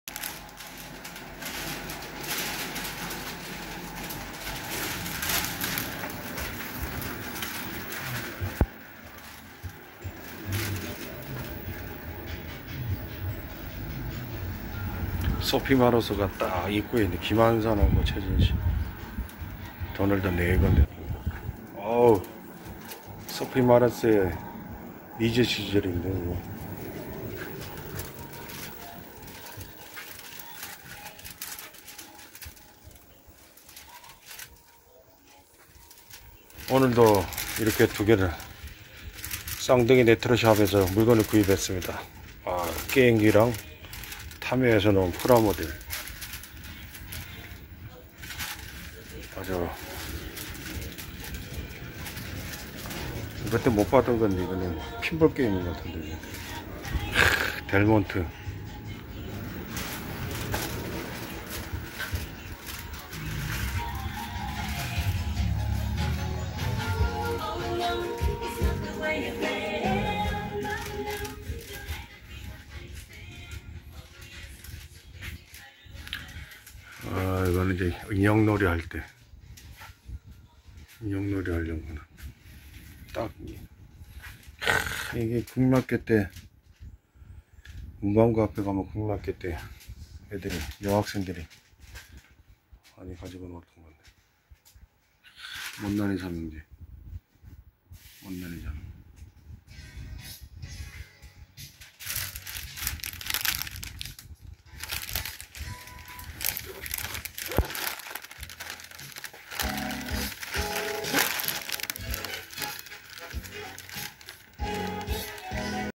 쌍둥이 레트로샵 방문
[ 쌍둥이 레트로샵 ]
서울시 동대문구 신설동에 위치하며 서울풍물시장과 동대문도서관의 주변에 있는 레트로샵이다.
오픈한지는 오래되지 않았으며 사장님과 많은 대화를 나누지는 못했지만 레트로관련 플리마켓행사장에서 만난적은 있었다. 최근에는 돈의문박물관마을에서 진행한 돈의문골목시장에서도 만났다.
동묘구제시장방향으로 가는 거리벼룩시장골목길에도 유사한 레트로샵이 있지만 규모는 작다.
신설동 서울풍물시장과 동묘벼룩시장을 자주 방문하였지만 쌍둥이 레트로샵은 처음 방문하여 설레이고 궁금함이 있었다. 가게는 건물의 2층에 위치하여 쉽게 찾을 수 있다. 참고로 E/L 는 없으며 3층에는 일반거주를 하고 있는 것 같다. 쌍둥이 레트로샵을 방문하여 전시된 다양한 레트로물건들을 관람하였다. 레트로물건들의 종류는 프라모델 , 완구 , 피규어 , 잡지 , 포스터 , 인형 , 잡화 , 문방구용품,비디오테이프 , 카세트플레이어등 다양한 과거 시대생활상을 반영한 다양한 레트로한 물건들이 많았다.